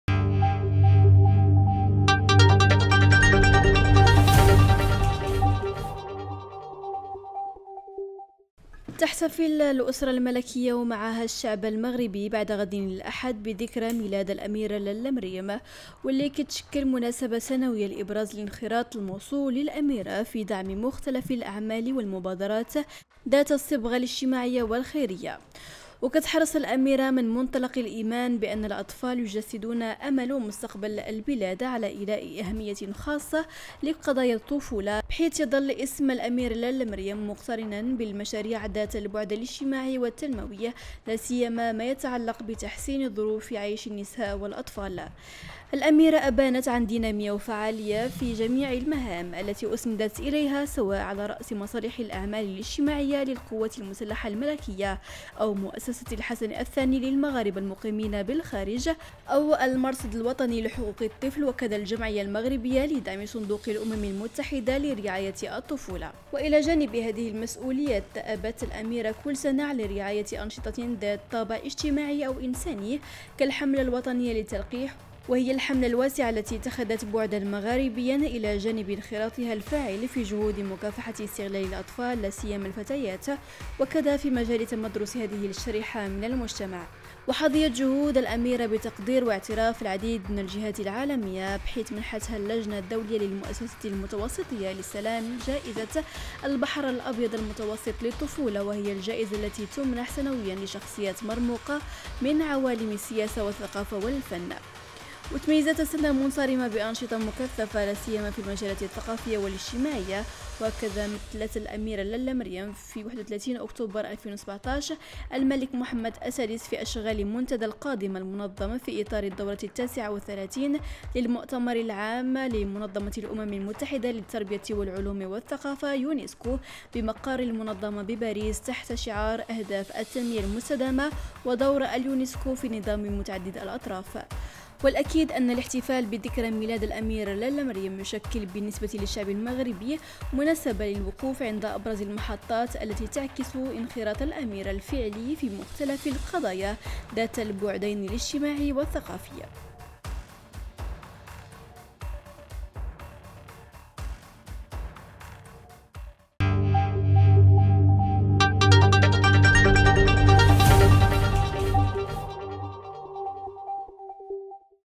مغاربة يحتفلون بذكرى ميلاد الأميرة للا مريم يوم الأحــد
تحتفل لأسرة الملكية ومعها الشعب المغربي، بعد غد الأحد، بذكرى ميلاد الأميرة للا مريم ، والتي تشكل مناسبة سنوية لإبراز الانخراط الموصول للأميرة في دعم مختلف الأعمال والمبادرات ذات الصبغة الاجتماعية والخيرية، وذلك على ضوء الرعاية الخاصة التي ما فتئت توليها سموها للفئات الاجتماعية الأكثر هشاشة، لاسيما النساء والأطفال.